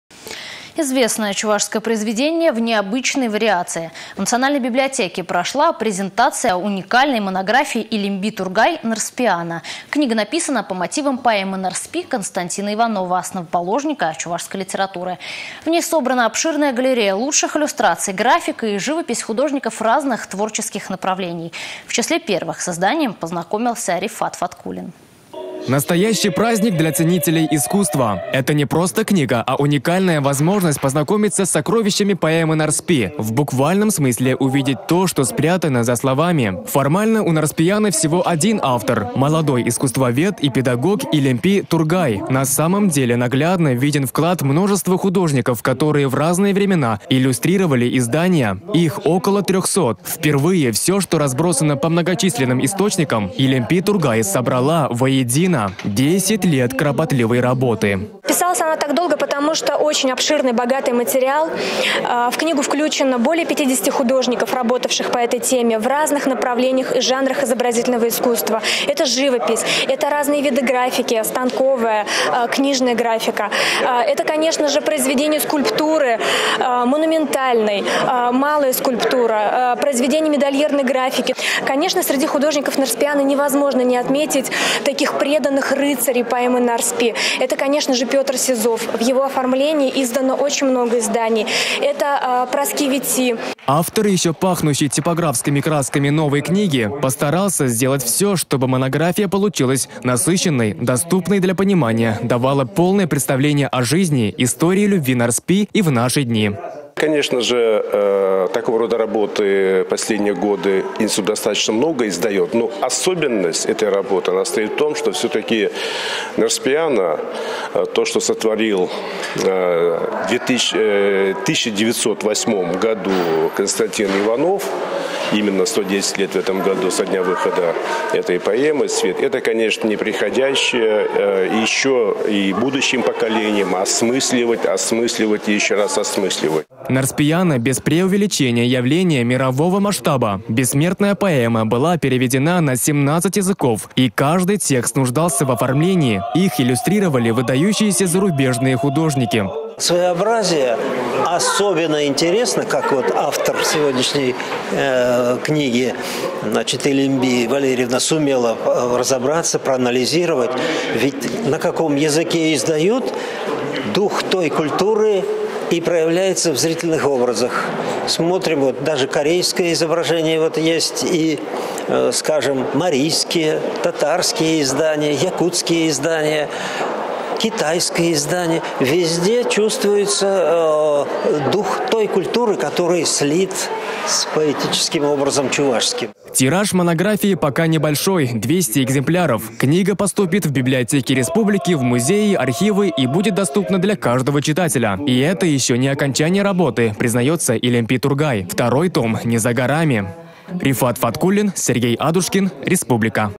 В Национальной библиотеке прошла презентация уникальной монографии Илемби Тургай «Нарспиана»
Книга написана по мотивам поэмы «Нарспи» Константина Иванова, основоположника чувашской литературы. В ней собрана обширная галерея лучших иллюстраций, графика и живопись художников разных творческих направлений. В числе первых с изданием познакомился Рифат Фаткуллин.